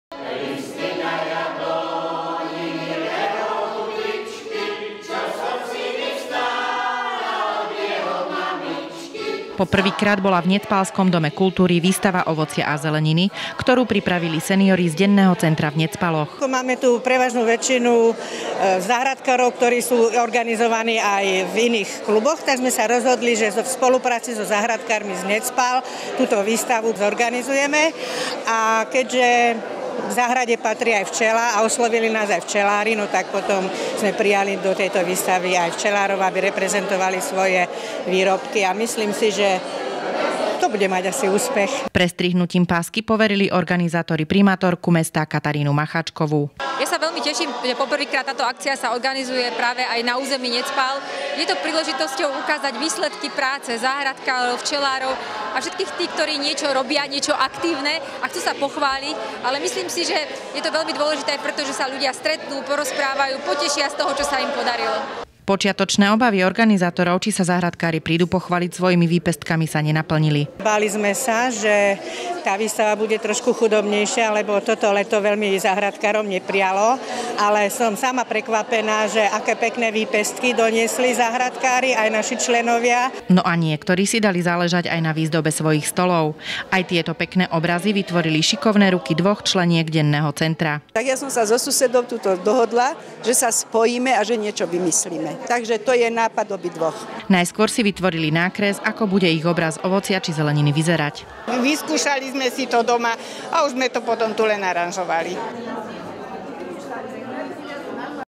Senior záhradkár
Seniori z denného centra v Necpaloch sa dali dokopy so záhradkármi a Regionálnym spolkom včelárov v Prievidzi, aby pripravili výstavu ovocia, zeleniny a ochutnávku medu. Návštevníci si mohli túto výstavu pozrieť počas dvoch dní v Dome kultúry v Necpaloch.